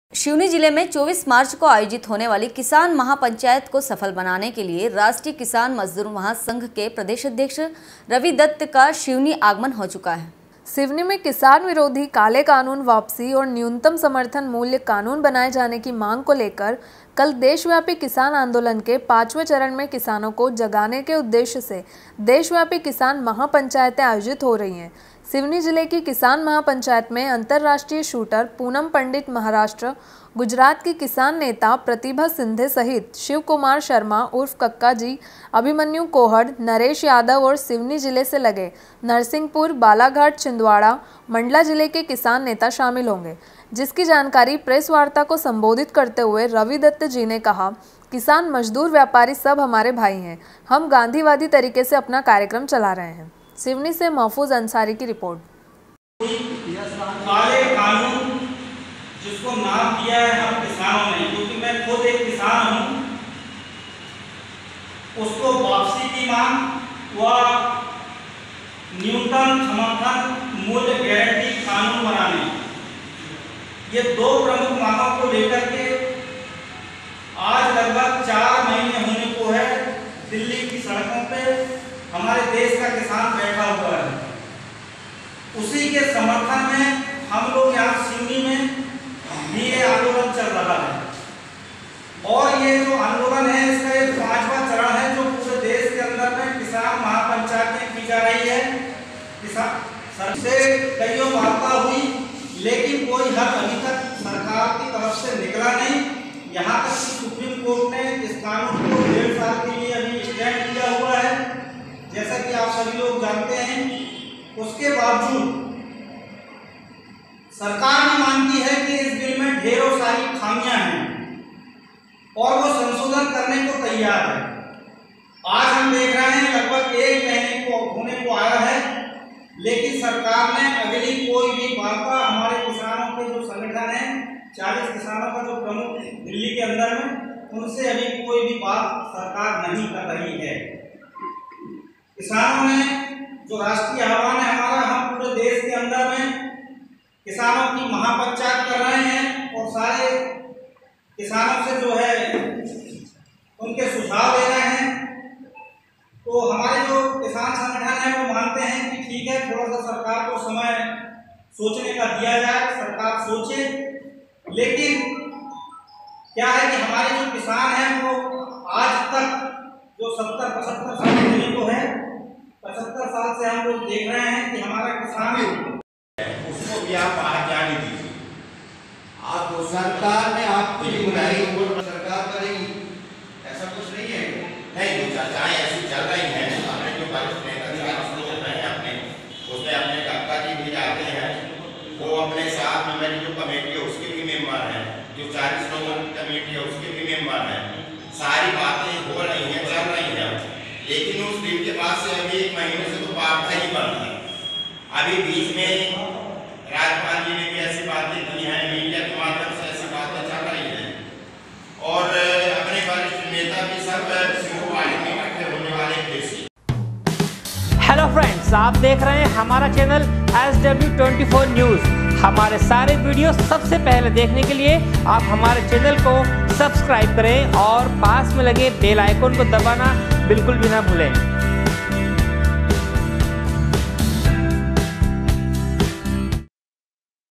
sivni- सिवनी जिले में 24 मार्च को आयोजित होने वाली किसान महापंचायत को सफल बनाने के लिए
सिवनी जिले में 24 मार्च को आयोजित होने वाली किसान महापंचायत को सफल बनाने के लिए राष्ट्रीय किसान मजदूर महासंघ के प्रदेश अध्यक्ष  रवि दत्त का सिवनी आगमन हो चुका है । सिवनी से महफूज अंसारी की रिर्पोट